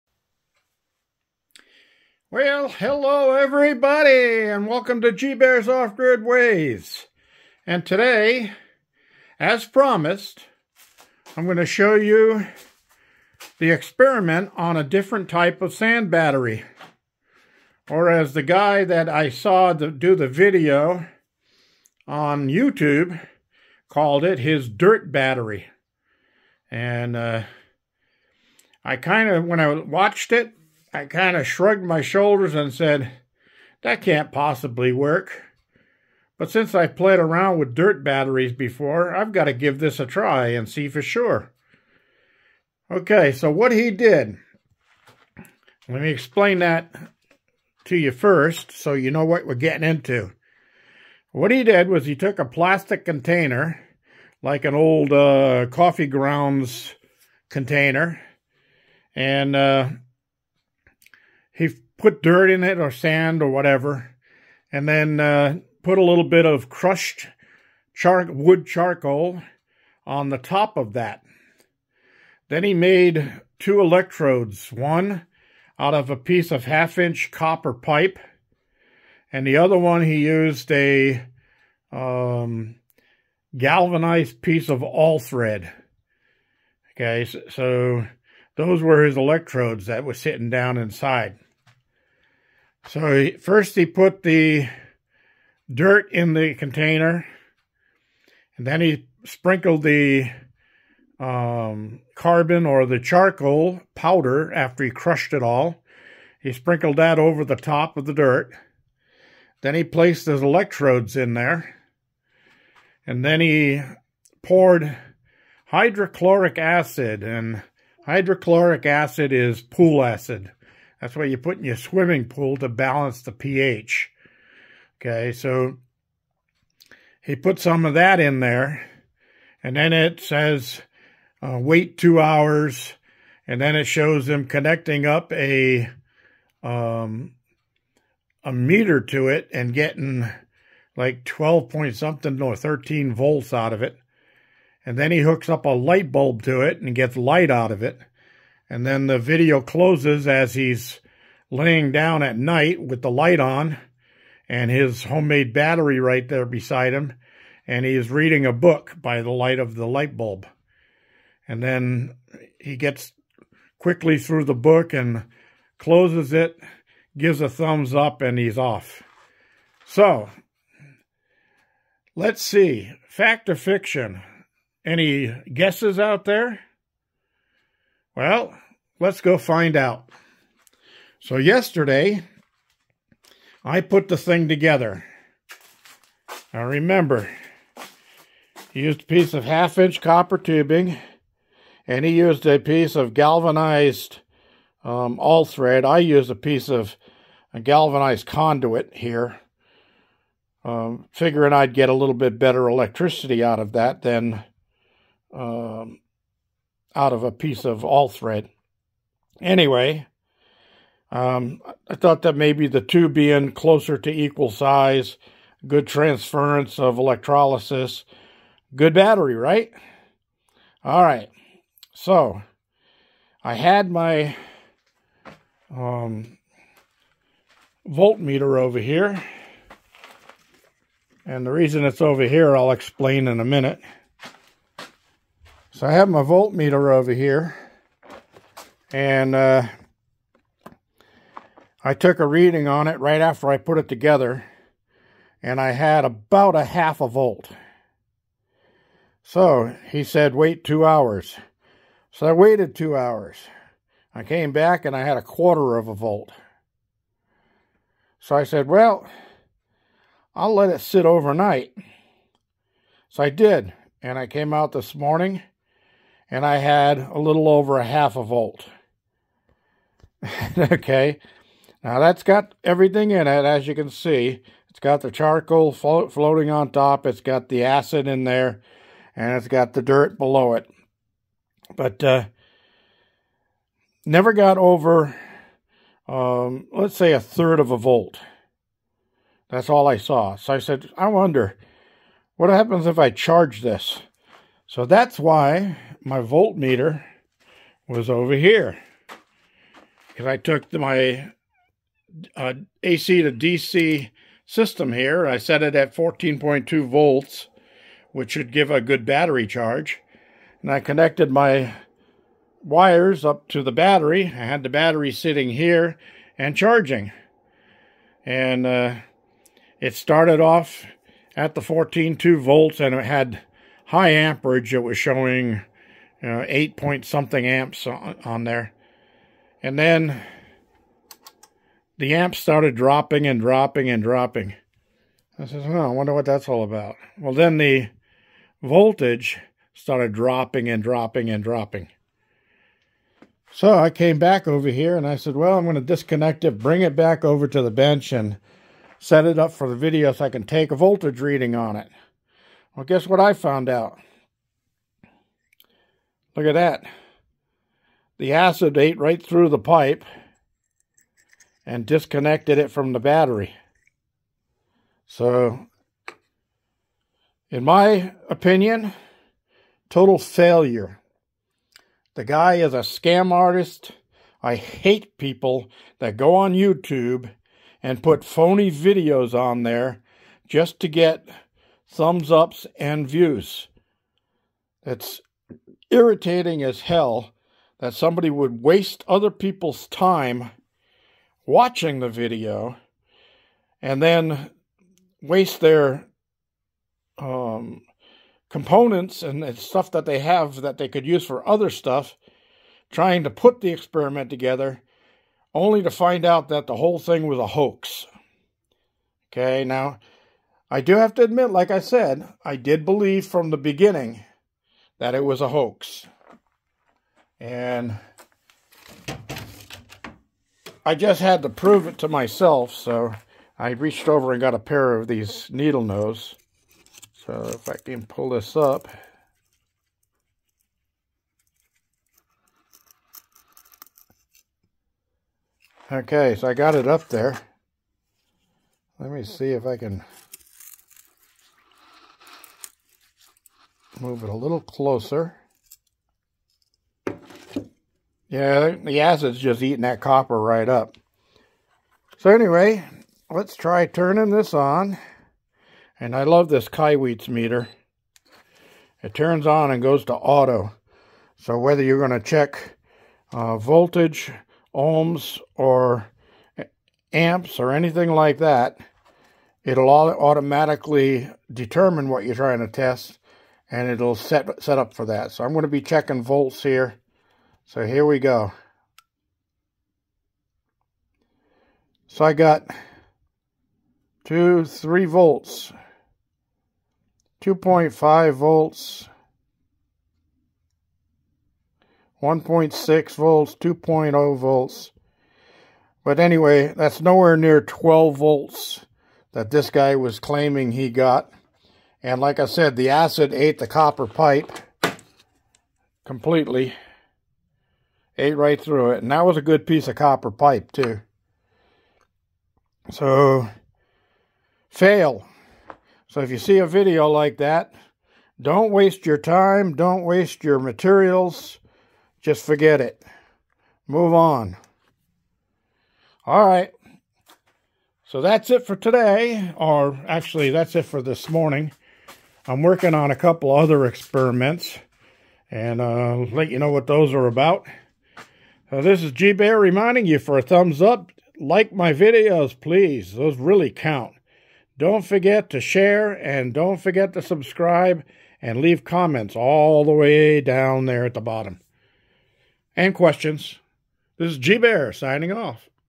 Another Type Of Sand/Dirt Battery? Nov. 18, 2023
Trying a battery as seen on YouTube. Will it work or not?